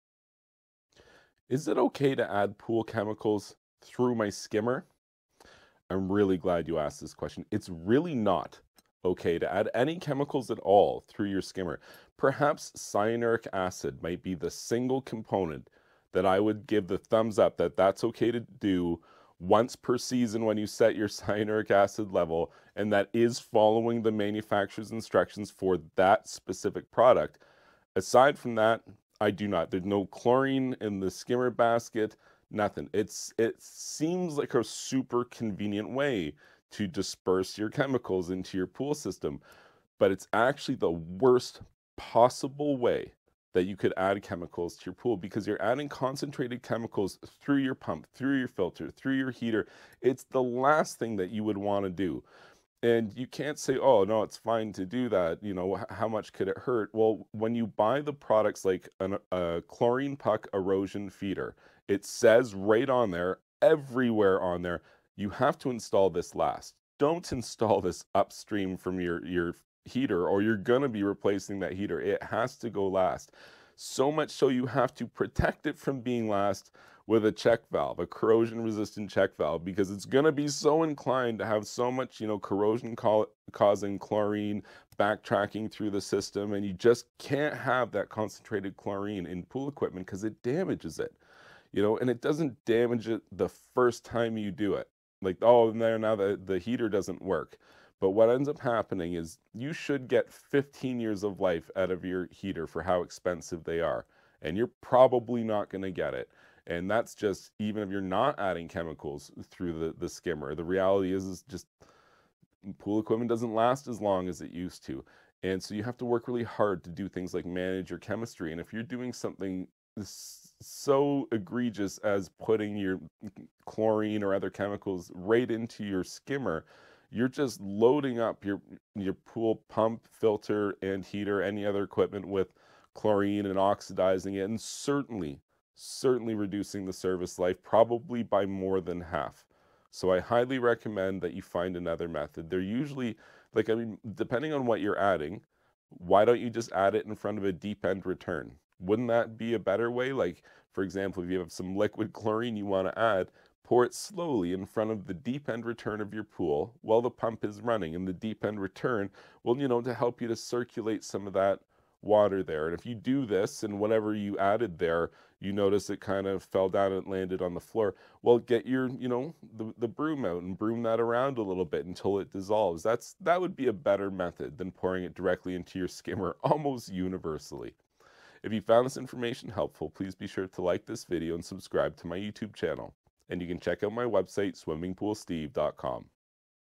Adding Chemicals Into Pool Skimmer
this video talks about the importance of never adding pool chemicals though your skimmer in order to protect both the skimmer itself from damage but also the rest of your filtration equipment as well.  Chlorine will reduce the service life for any plastic that it contacts, as well as metals within items like a pool heater.  The rate of deterioration is very fast and if you add chemicals through your pool skimmer regularly you should expect to experience early equipment failure for all of your filtration equipment.  Chlorine should only be added through an erosion feeder or by manually adding to the pool...being sure to avoid adding it through the skimmer.